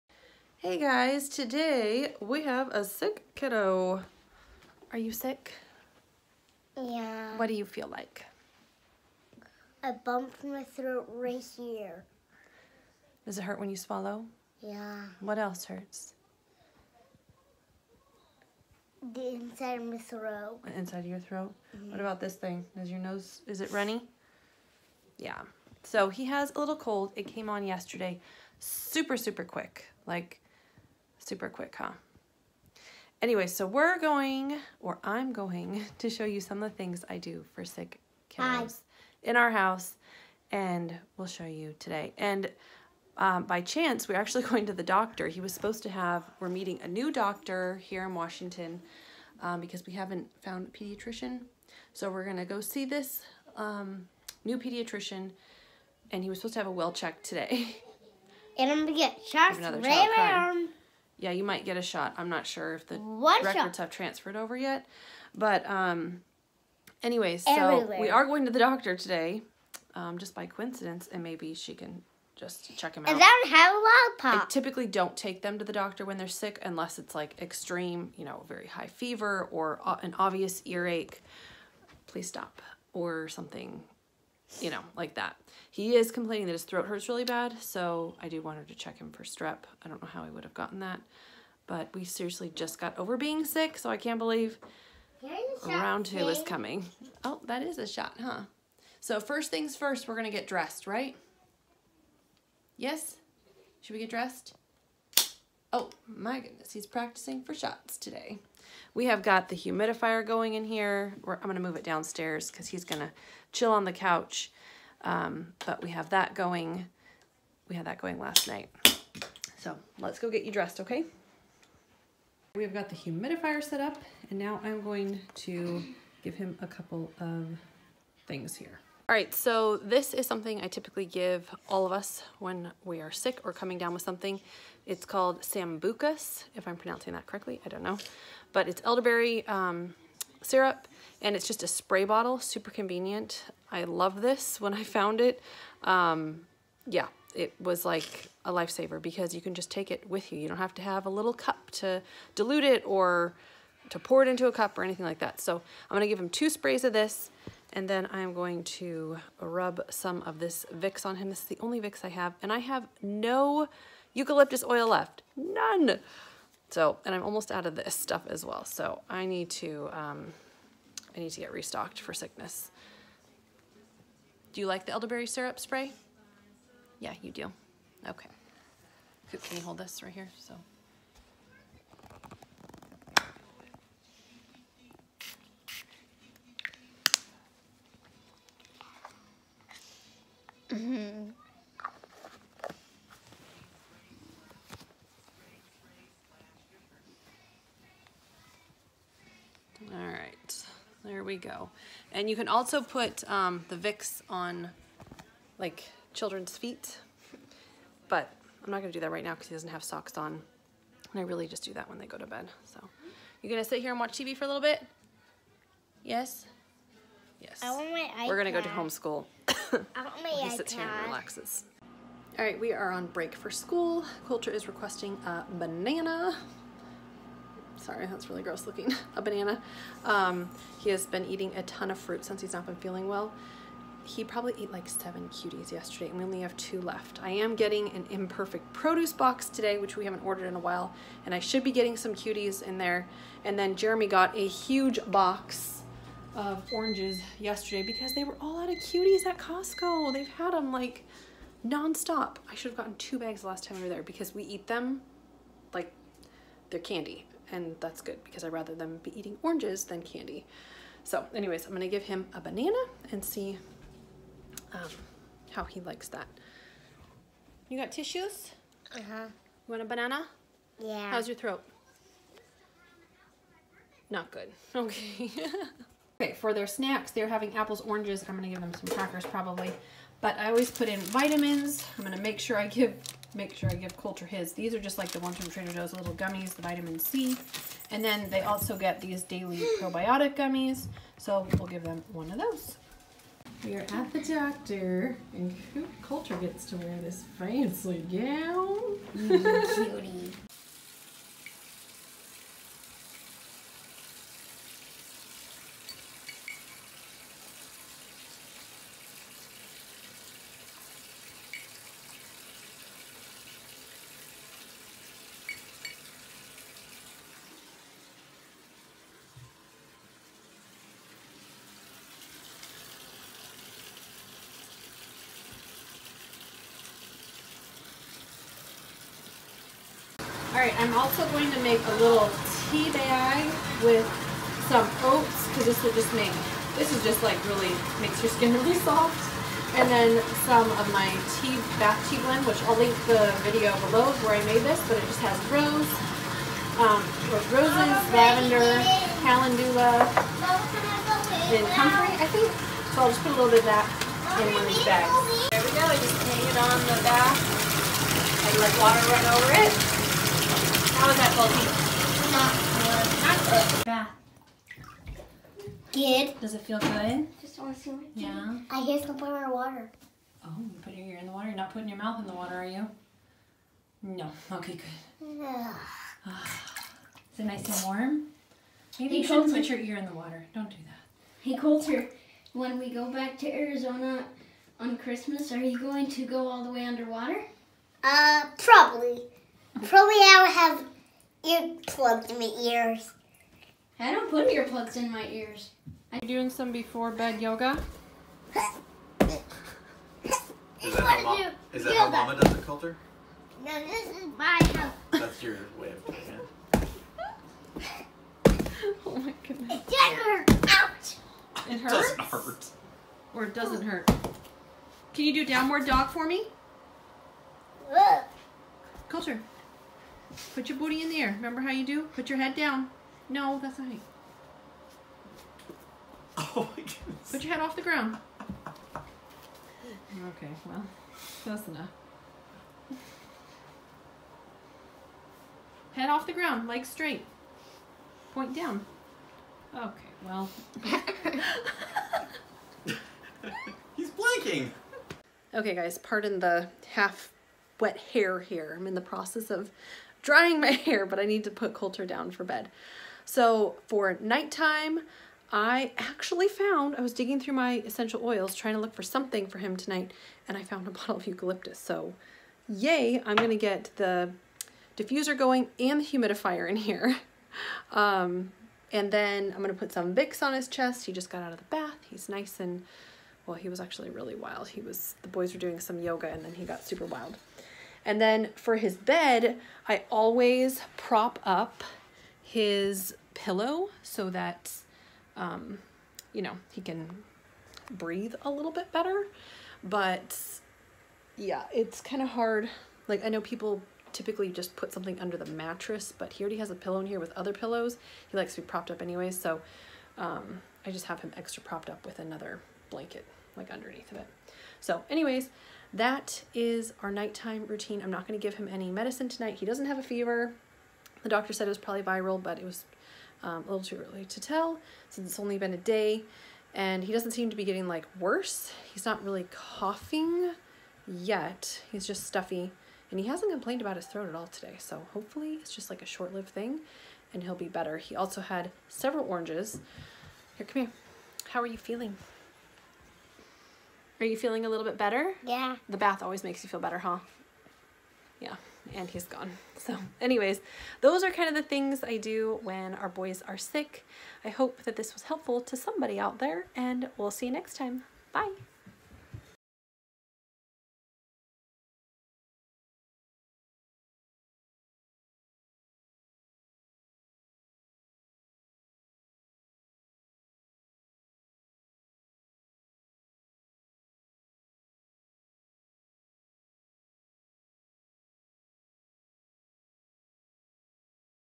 DITL With A Sick Kiddo || What We Do To Treat The Common Cold
I would love to hear your natural cures for colds and viruses. Please share below.